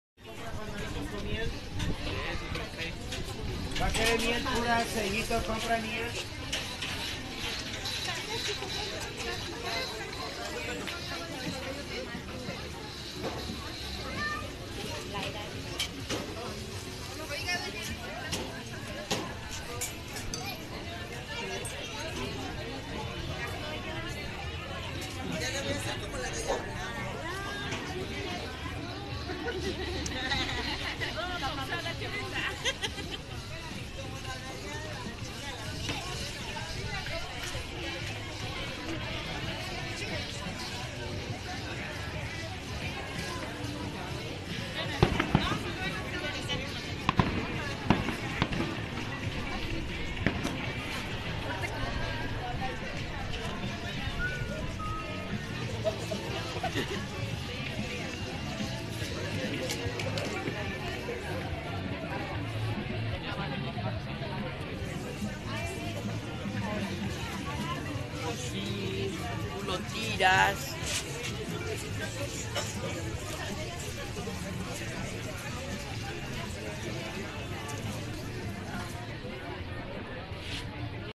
Festival del Pulque y el corredor gastronómico en el parque intermunicipal de San Andrés Cholula
Así el Festival del Pulque y el corredor gastronómico en el parque intermunicipal de San Andrés Cholula, visitantes locales y de otras partes del país como el extranjero se dan cita en torno a la zona arqueológica de Cholula.